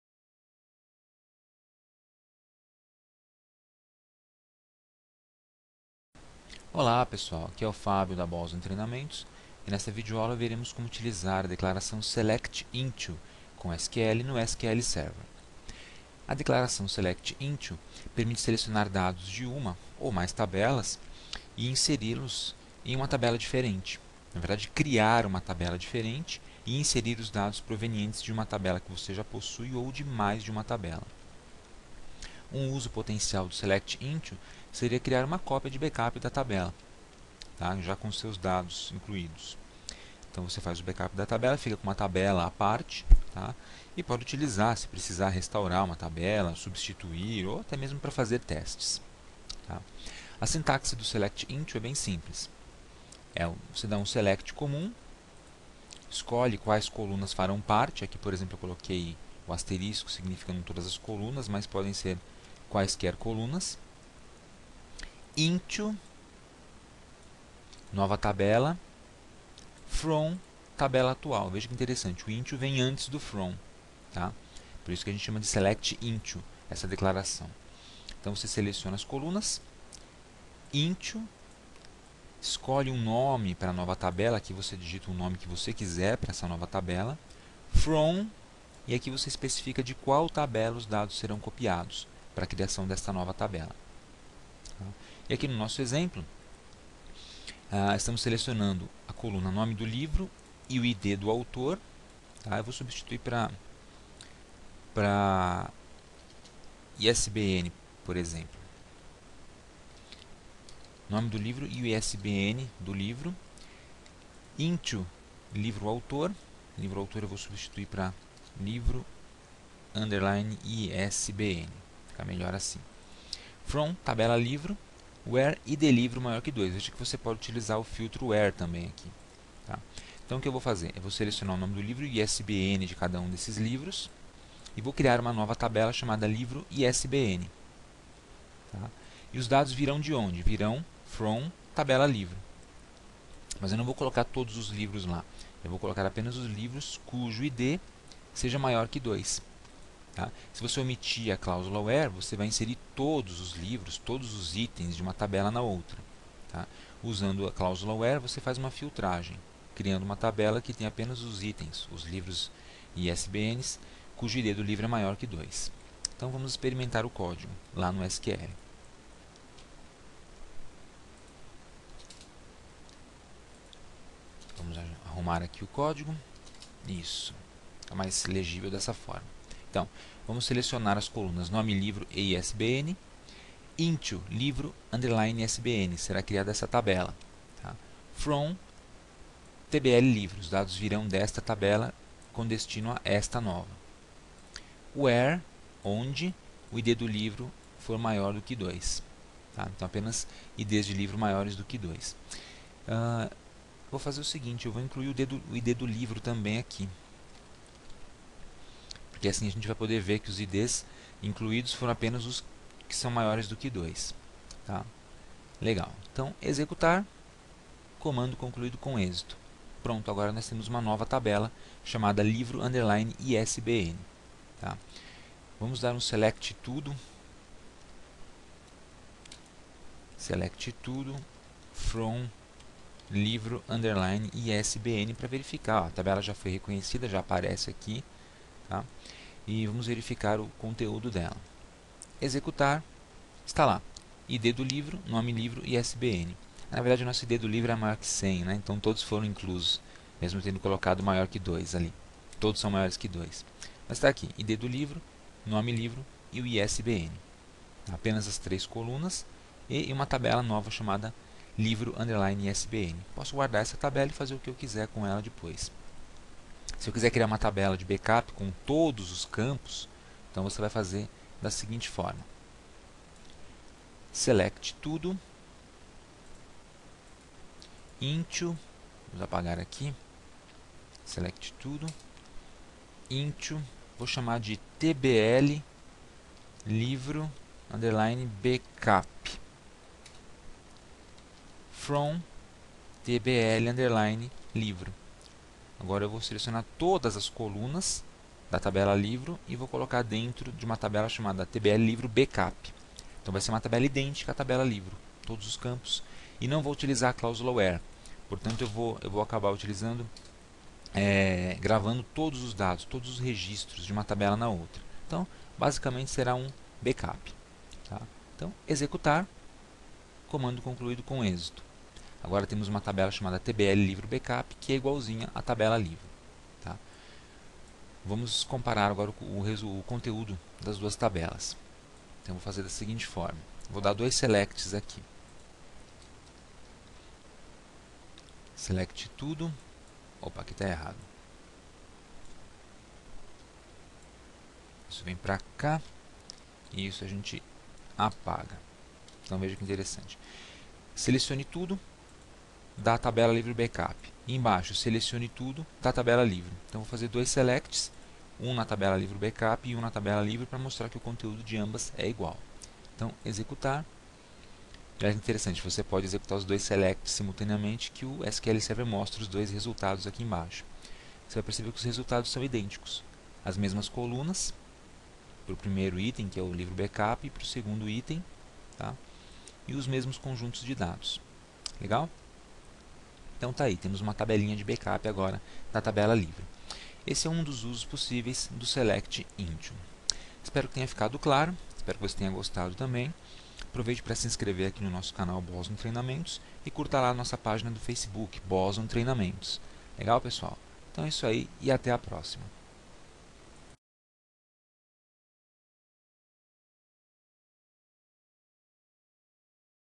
18 - T-SQL - SELECT INTO - Criar nova tabela a partir de uma tabela existente - SQL Server
Curso de SQL Server - Declaração SELECT INTO - Como criar uma nova tabela a partir de uma tabela existente no SQL Server